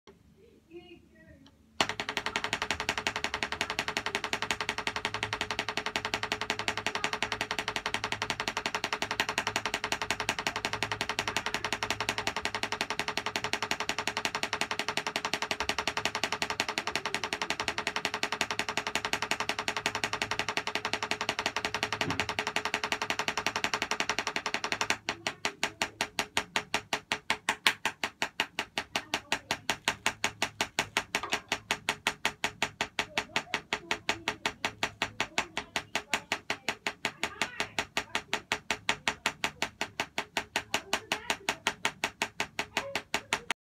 Me practicing another single Stroke Roll and right hand drum sticking exercise at 160bpm
This is another single stroke Roll and right hand drum sticking exercise, it is fun to do, I enjoyed doing it, and i liked doing it. Enjoy.